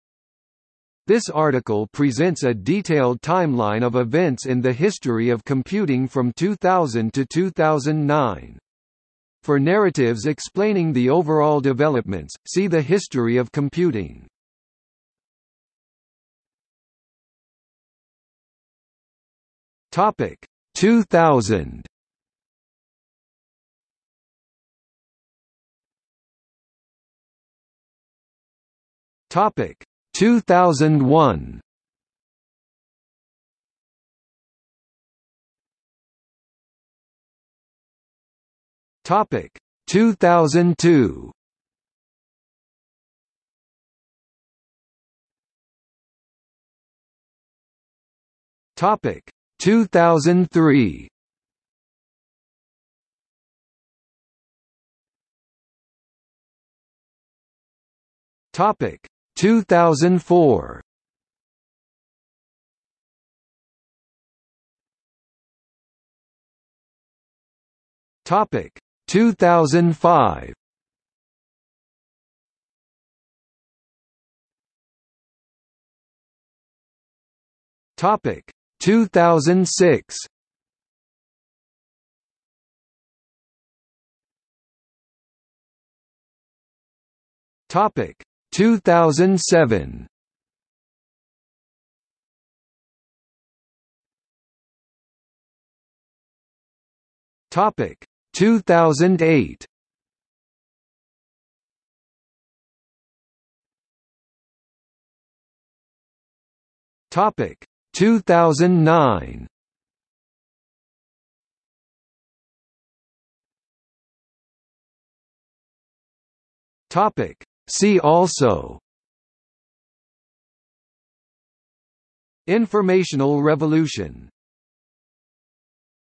Timeline of computing 2000–09 | Wikipedia audio article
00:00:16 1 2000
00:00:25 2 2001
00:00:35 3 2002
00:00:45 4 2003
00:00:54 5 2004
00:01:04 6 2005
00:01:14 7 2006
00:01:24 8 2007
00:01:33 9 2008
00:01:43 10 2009
00:01:53 11 See also

- Socrates

SUMMARY
This article presents a detailed timeline of events in the history of computing from 2000 to 2009. For narratives explaining the overall developments, see the History of computing.